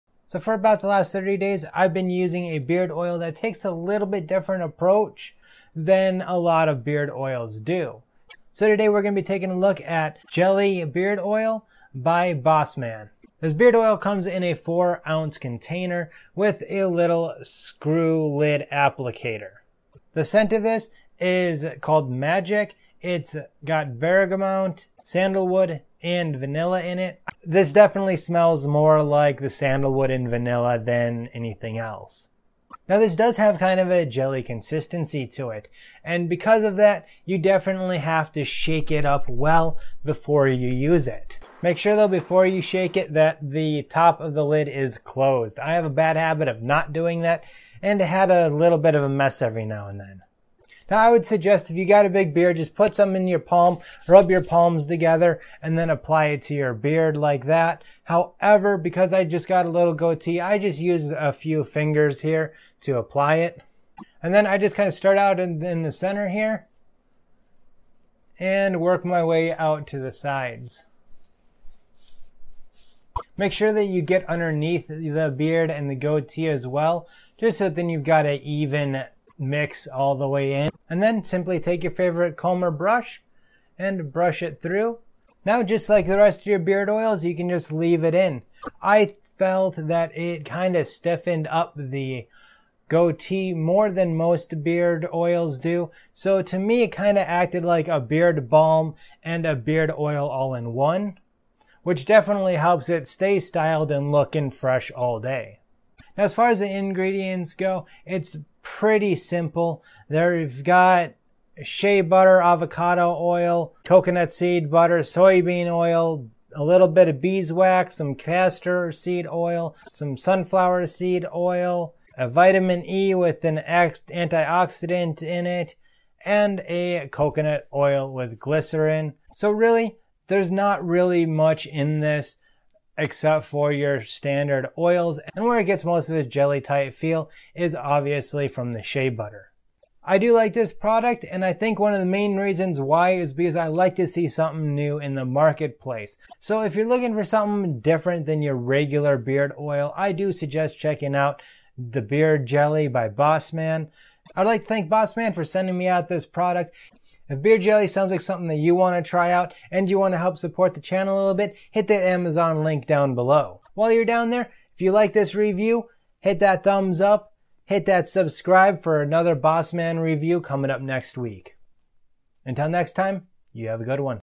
Jelly Beard Oil by Bossman
Jelly Beard Oil by Bossman is a much different take on beard care. Bossman jelly combines beard oil with a beard balm that helps hold your beard in place with a jelly as well as give your hair and skin all the nutrients it needs through the oils.

all scents kit=